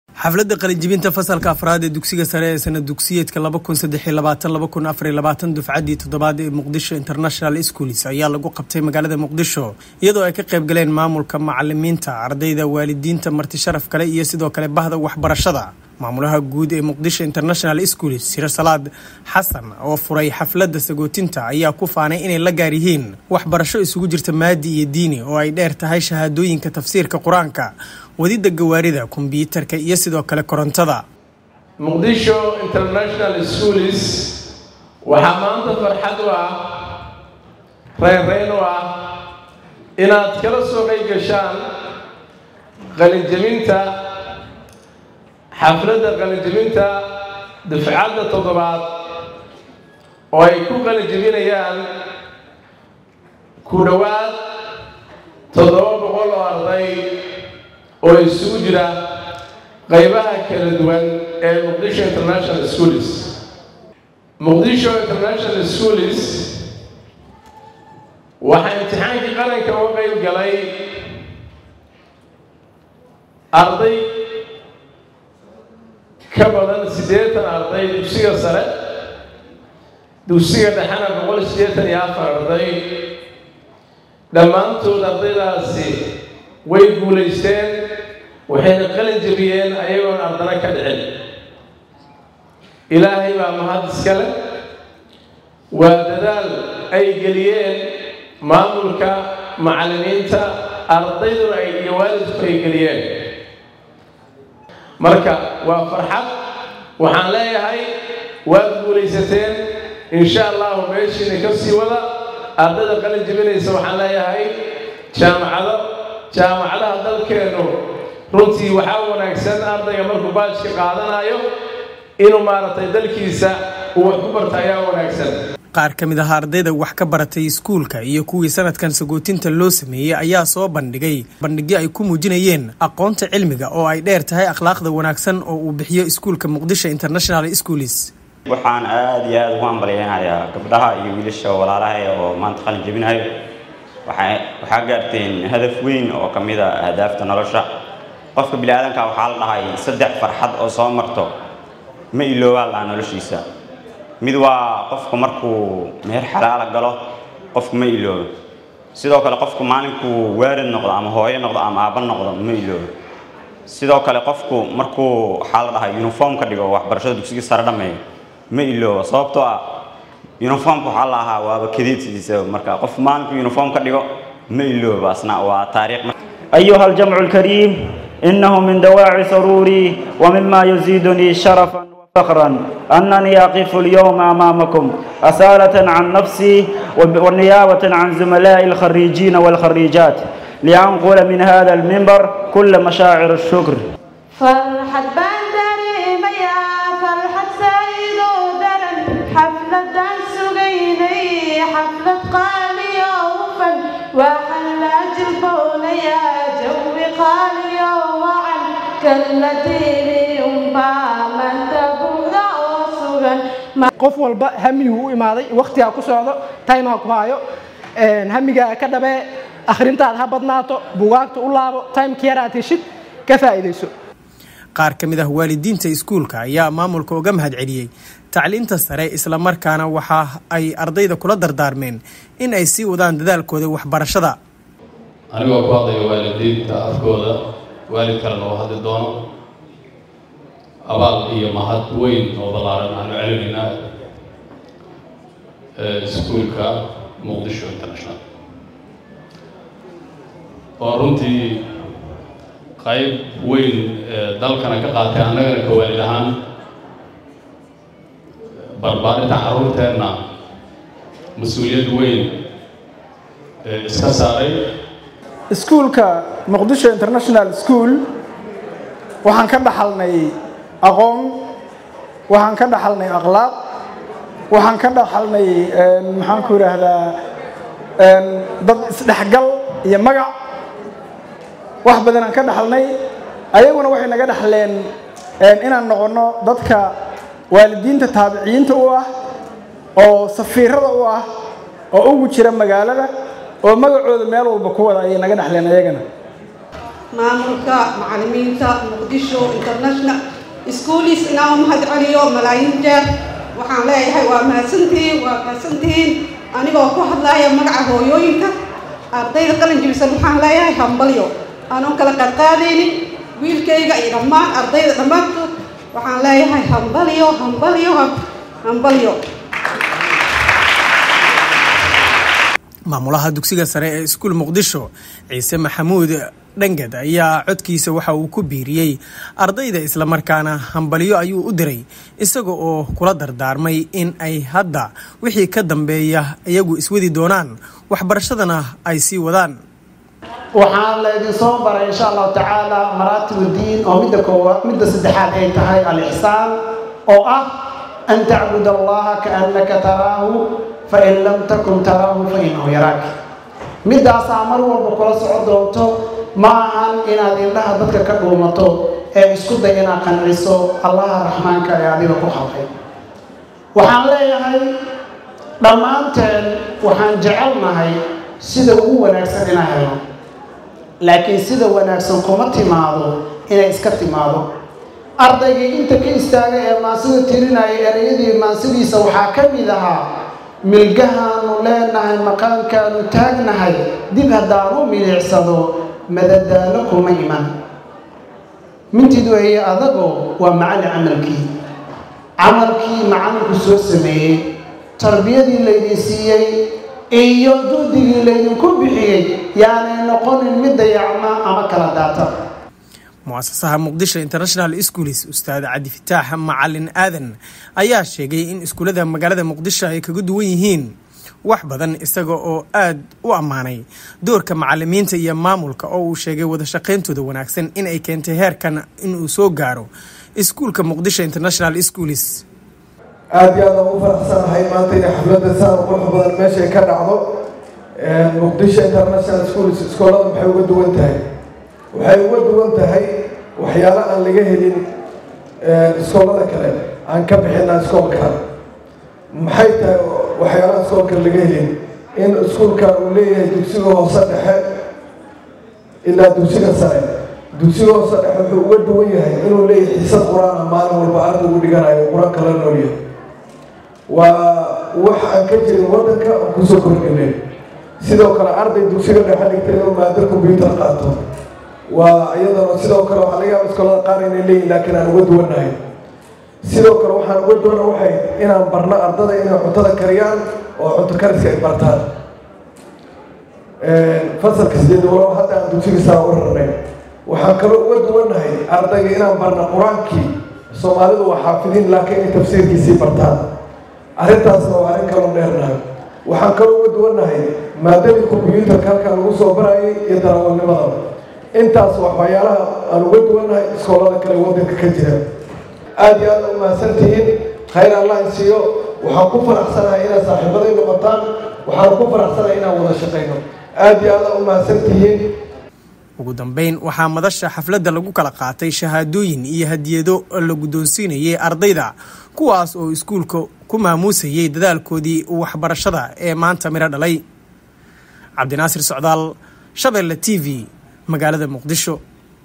Mogadishu international schools oo maanta xaflad sagootin ah u sameeyay dufcadii 7aad ee............
Mogadishu international schools oo maanta xaflad sagootin ah u sameeyay dufcadii 7aad ee dugsiga sare ee sanad dugsiyeedka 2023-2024.